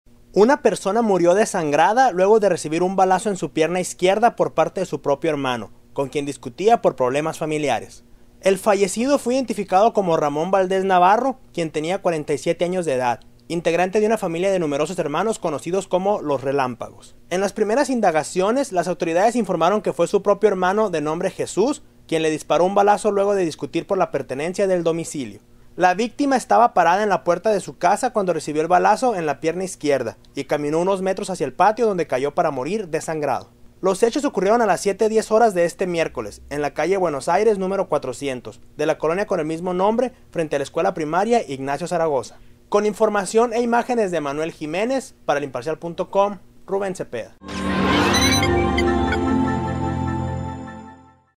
Asesina hombre a su hermano en Nogales
Una persona murió desangrada luego de recibir un balazo en su pierna izquierda por parte de su propio hermano con quien discutía por problemas familiares.